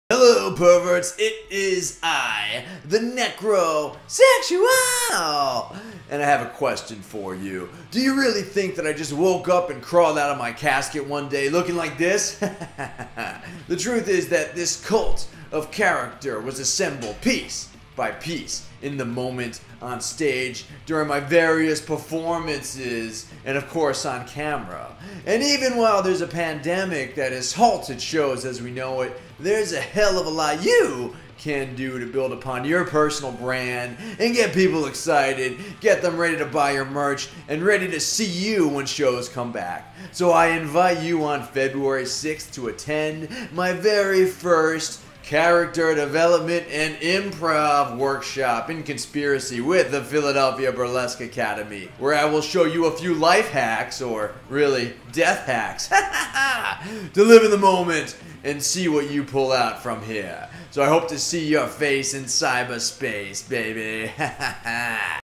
Necrosexual's Cult Of Character Virtual Workshop
Less than a week away to  learn character development and improv from the showghoul who ain't nobody's fool! February 6 at 1 pm EST via Zoom Conference.